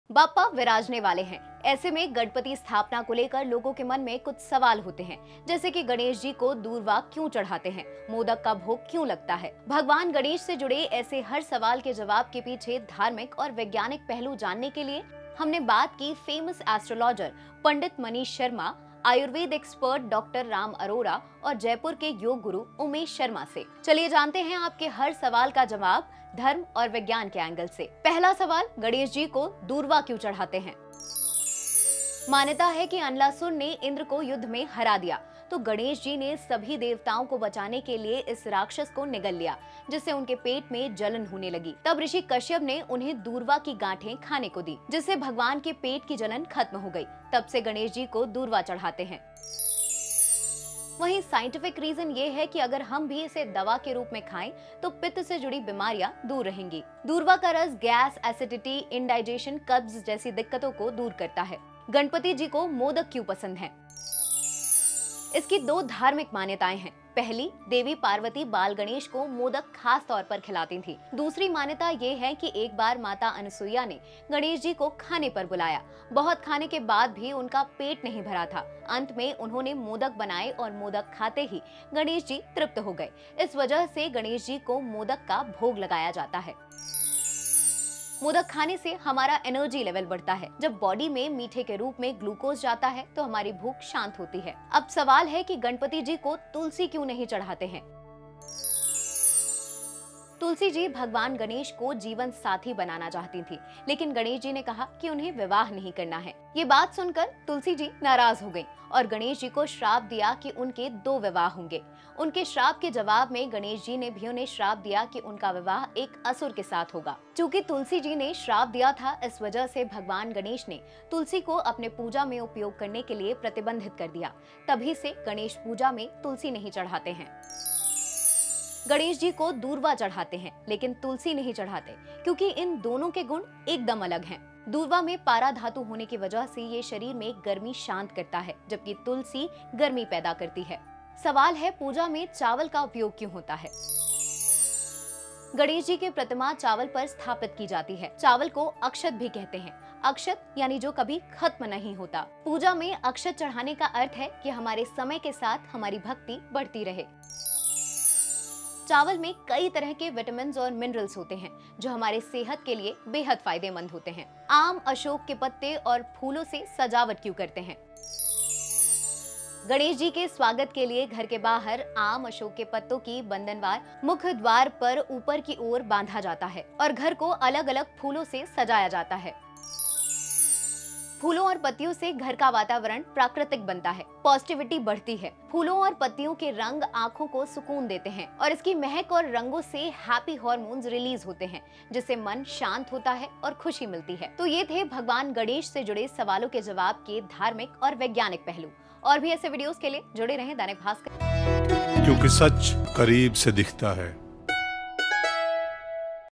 गणपति पूजा का साइंस
गणपति पूजा का साइंस: गणेश को दूर्वा चढ़ाने से कलश पूजा करने तक की 10 परंपराएं, इनके पीछे है धर्म के साथ विज्ञान भी